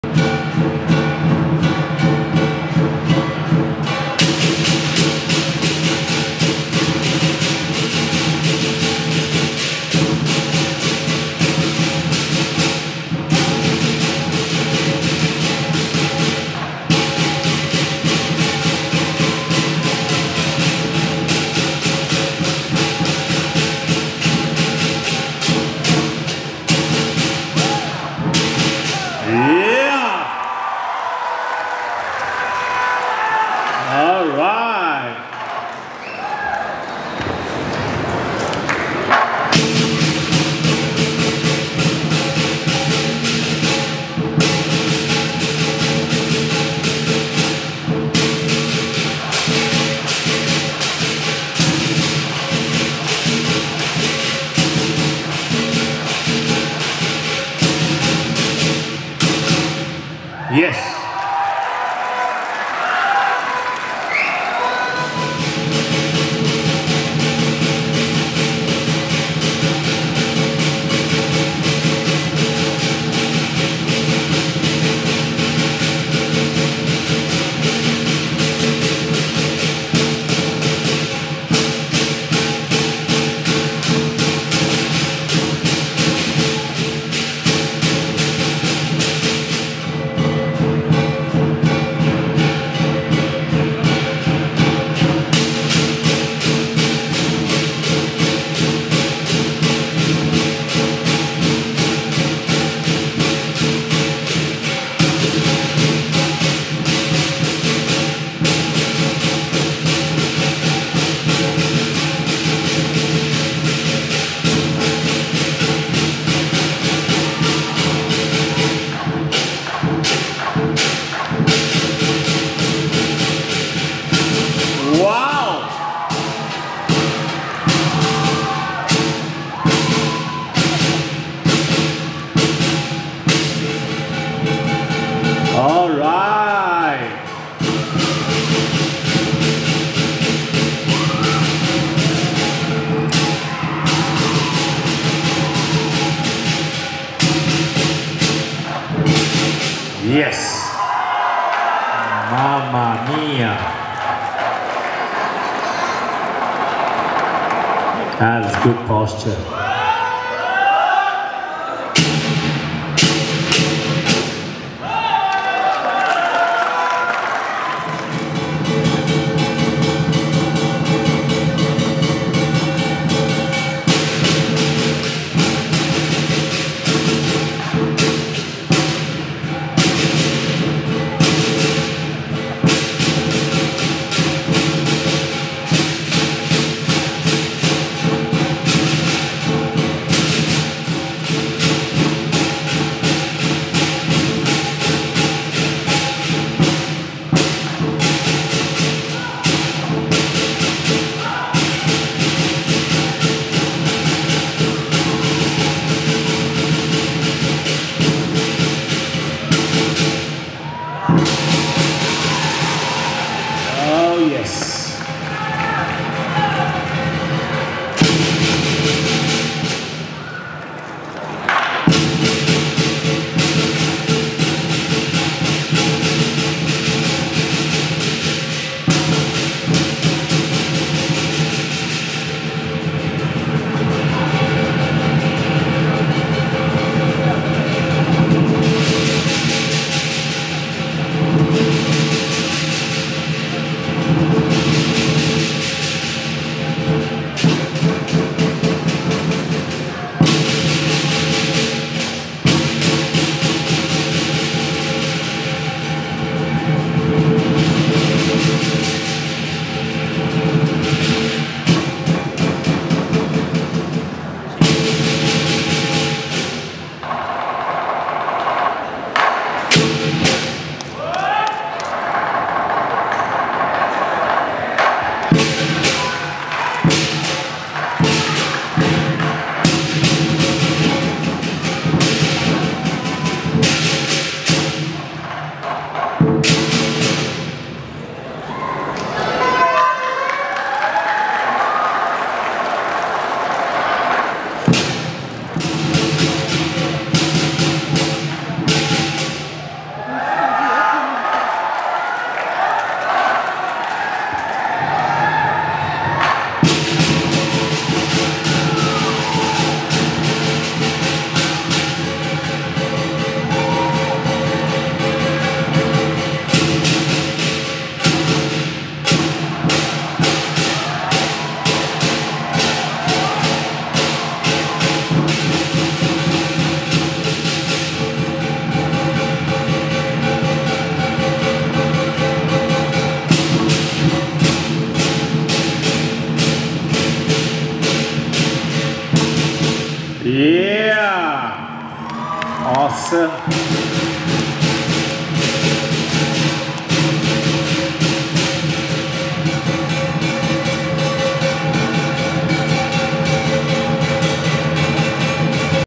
Fearless lion with power waist
An exciting acrobatic lion dance on high poles by Persatuan Tarian Singa Lee Kiong (沙巴李强龙麒狮团), a team with strong waist that is capable of doing many difficult moves.